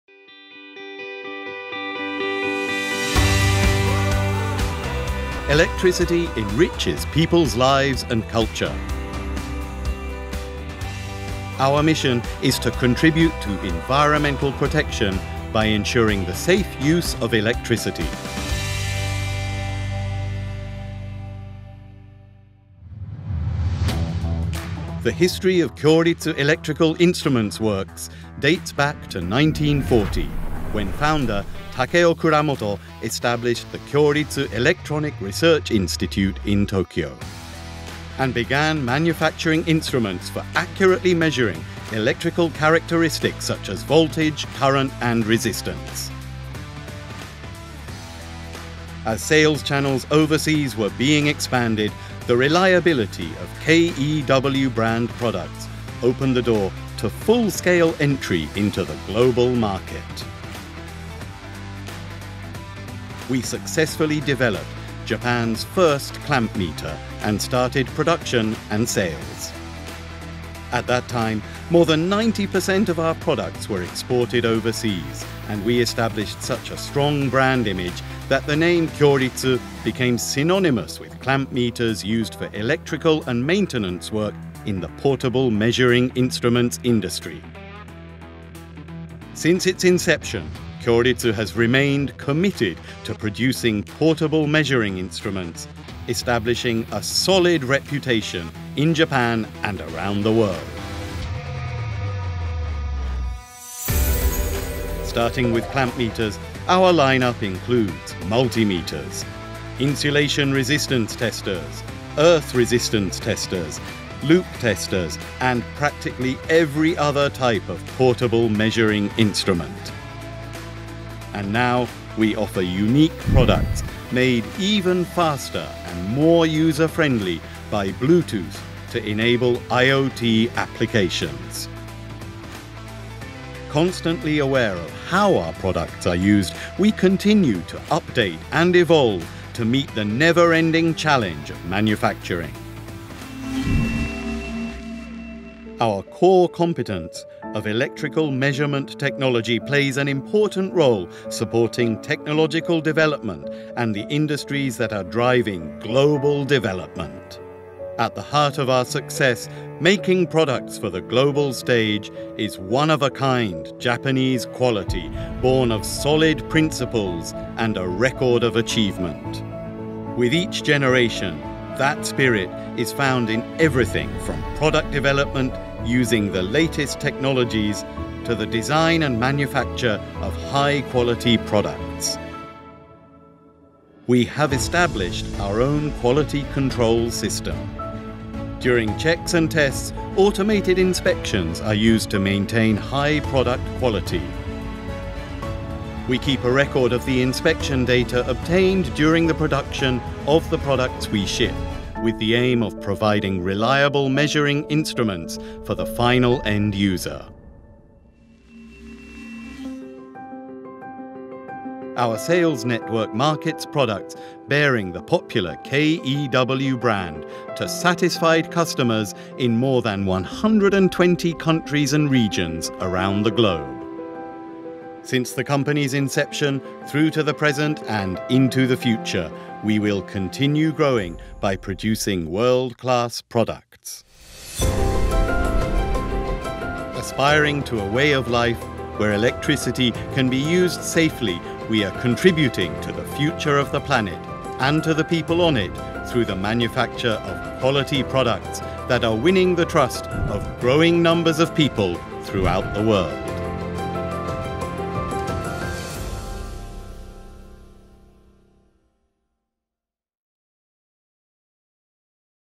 KYORITSU Company Profile Video
KYORITSU ELECTRICAL INSTRUMENTS WORKS, LTD.

Products
Multimeters, Voltage Testers, Clamp Meters, Insulation/ Continuity Testers,
Earth Testers, LOOP/PFC/PSC Testers, RCD Testers,
Power Quality Analyzer, Power Meters, Loggers, Clamp Sensors,
Phase Indicators, Illuminometers,Recorders, etc.

00:00 Overview
00:24 History
01:44 Products
02:26 Japan Quality
03:32 Global Network